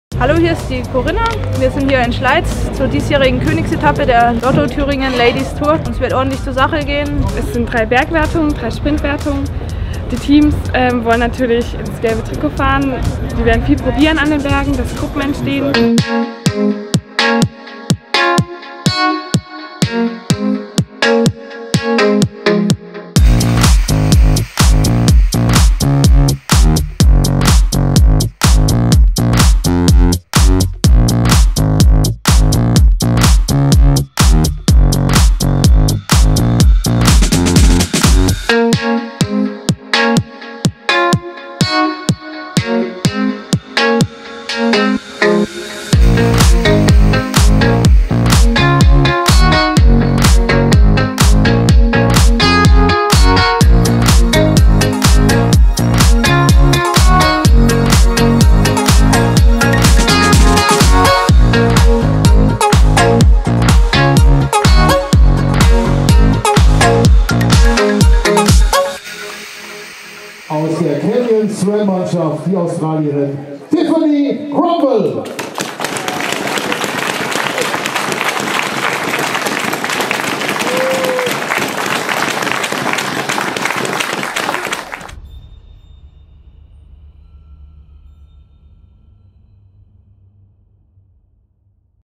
LTLT17 Schleiz
Schleiz is done 2 of 7!
The first and longest stage of this years’ tour ended with another stage win for Canyon Sram Racing.
Watch the video for all impressions.